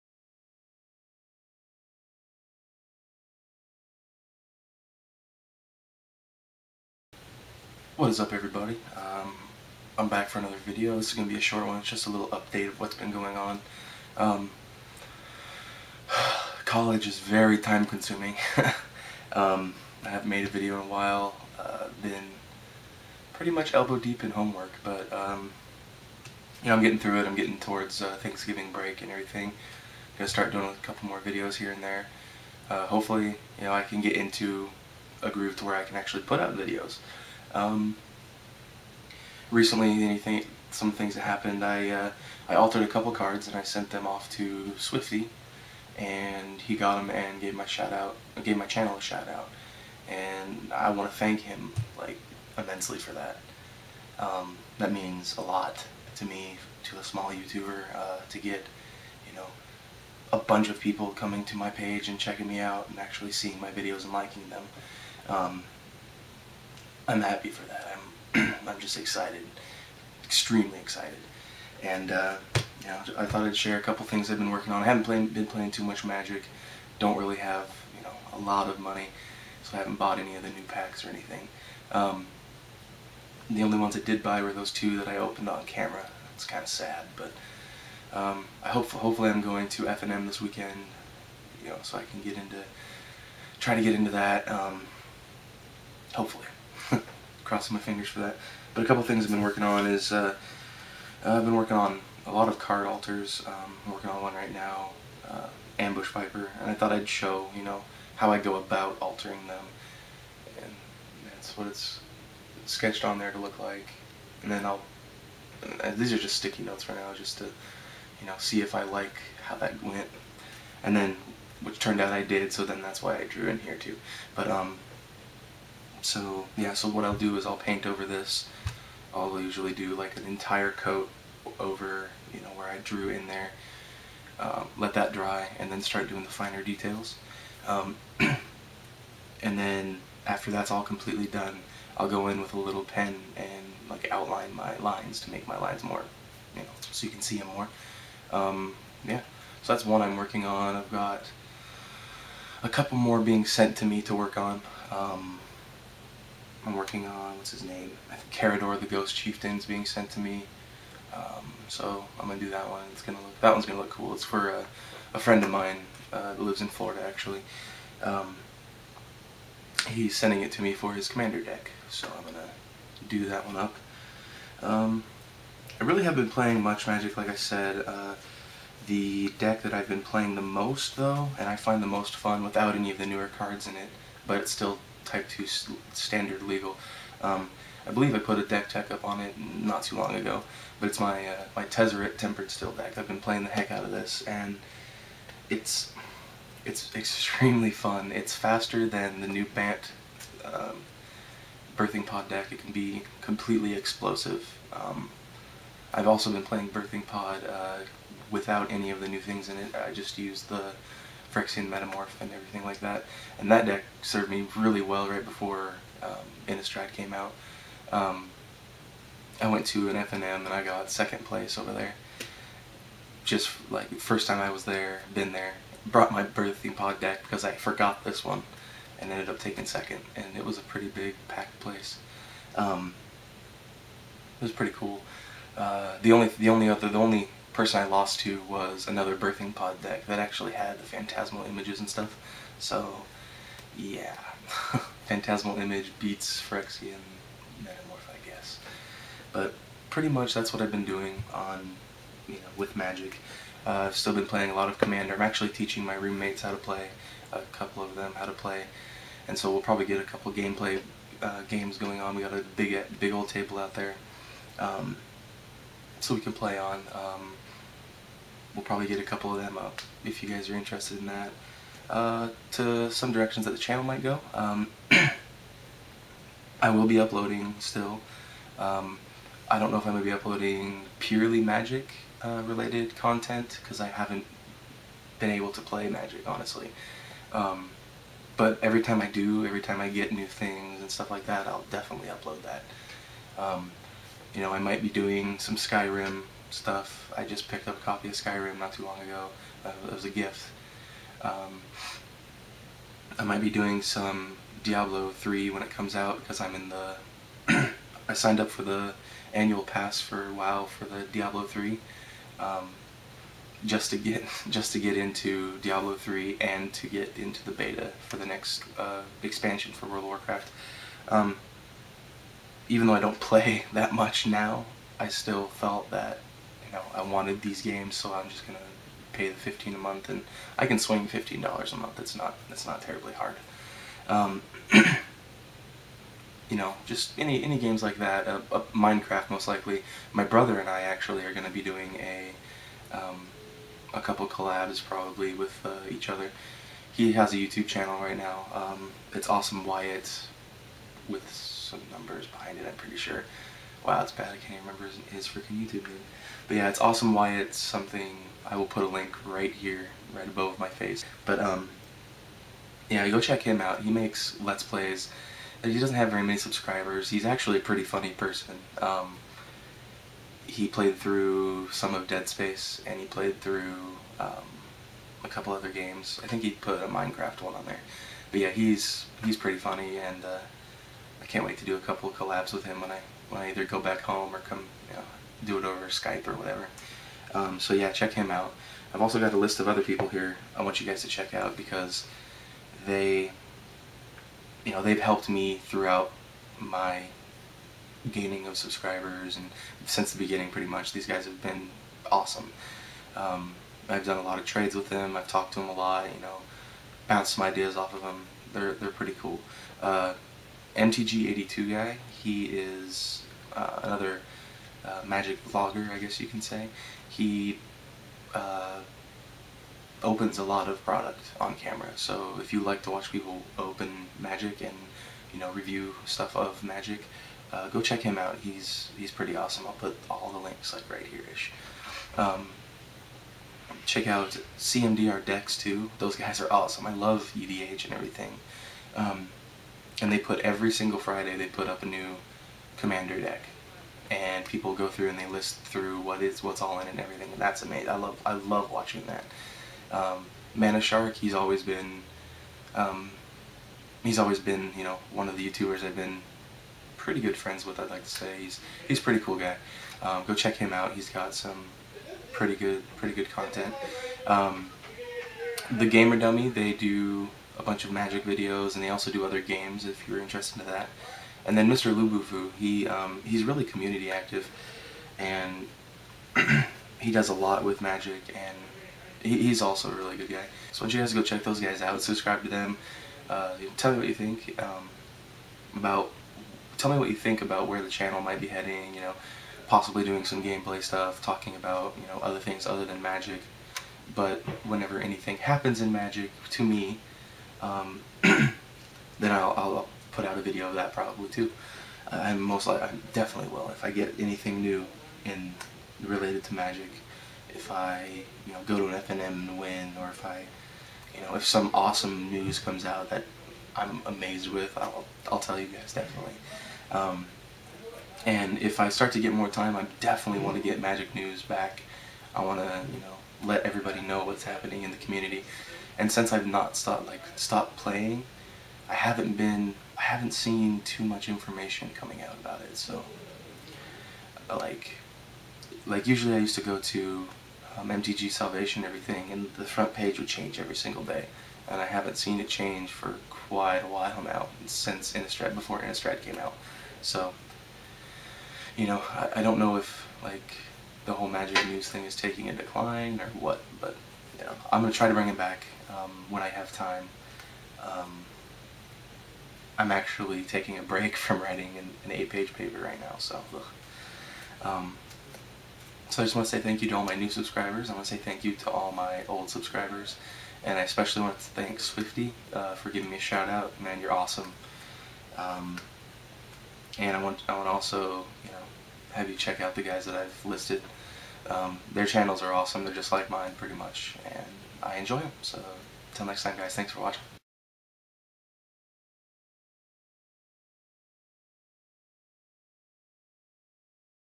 MTGMondays Episode #29 A Huge Thank You to Swifty and Some Updates!
In this episode I go over some updates to me and my channel and I give out Huge thanks to Swifty for the shout out